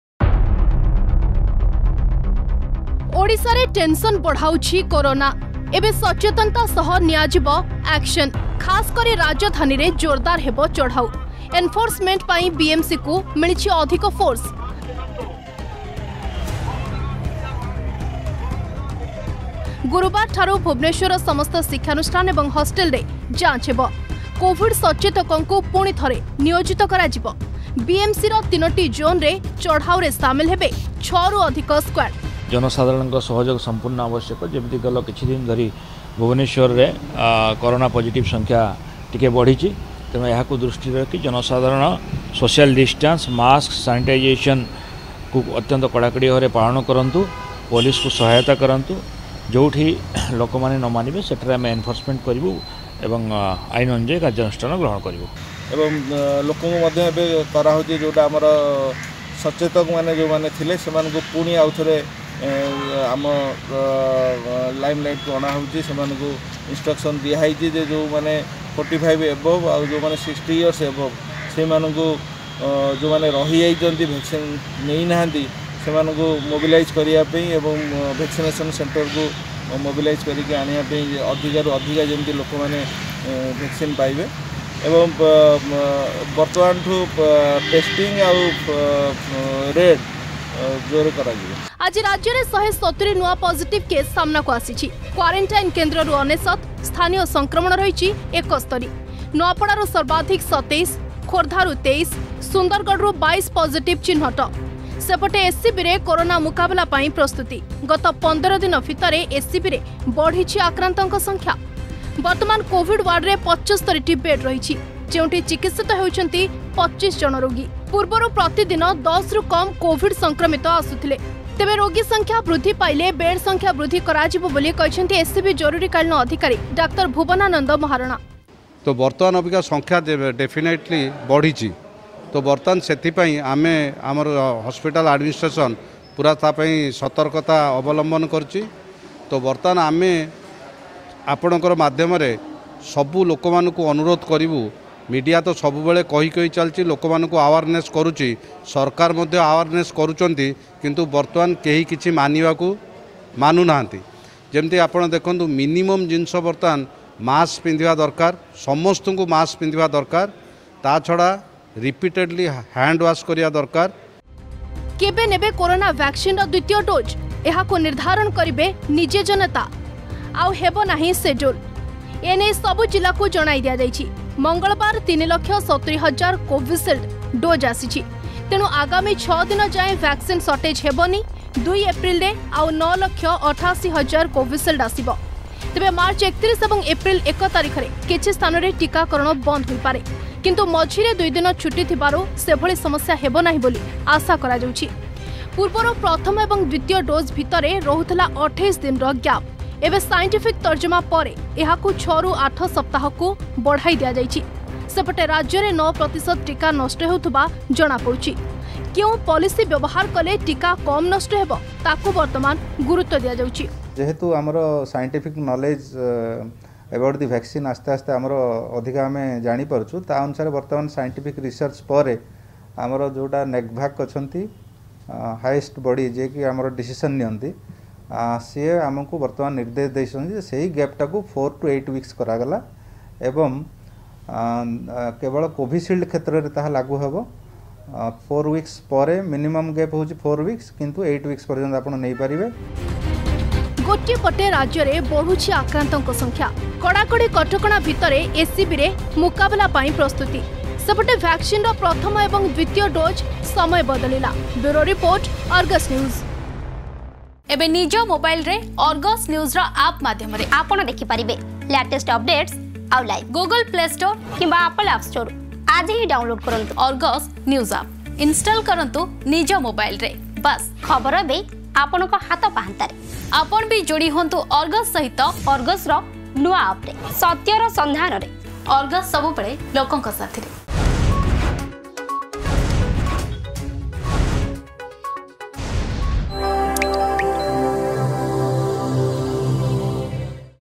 ସଚେତନ ନ ହେଲେ ଆକ୍ସନ
ରାଜ୍ୟରେ ଗୋଟିଏ ଦିନରେ ପୁଣି ଶହେ ପାର ପଜିଟିଭଙ୍କ ସଂଖ୍ୟା । 24 ଘଣ୍ଟାରେ 170 ଆକ୍ରାନ୍ତ ଚିହ୍ନଟ । ସେପଟେ ସଚେତନତା ସହ ଗାଇଡଲାଇନ ନ ମାନିଲେ ନିଆଯିବ ଆକ୍ସନ । ଅମାନିଆଙ୍କ ପ୍ରତି ବିଏମସି ଓ ପୋଲିସ ଅଧିକ କଠୋର ହେବ । ସେପଟେ କରୋନା ମୁକାବିଲା ପାଇଁ ପ୍ରସ୍ତୁତ ହେଉଛି ଏସସିବି ମେଡିକାଲ.